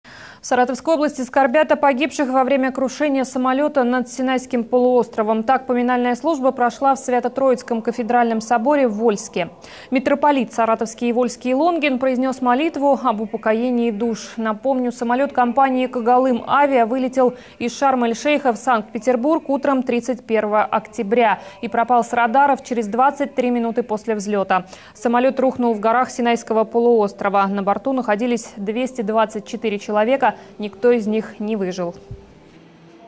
В Саратовской области скорбят о погибших во время крушения самолета над Синайским полуостровом
Так, поминальная служба прошла в Свято-троицком
кафедральном соборе в Вольске. Митрополит Саратовский и и Вольский Лонгин произнес молитву об упокоении душ. Материал вышел в эфир 2 ноября 2015 года.